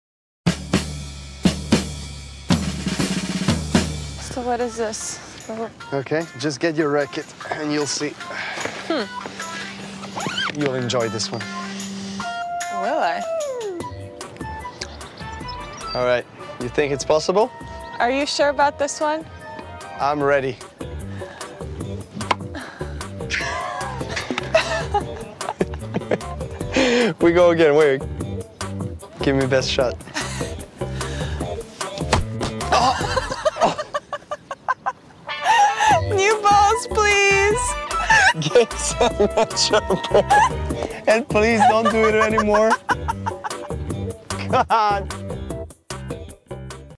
(Head) Maria Sharapova Hit The Nuts Of Novak Djokovic
(HEAD) speed vs instinct sharapova hit the nuts of djokovic sharpshooting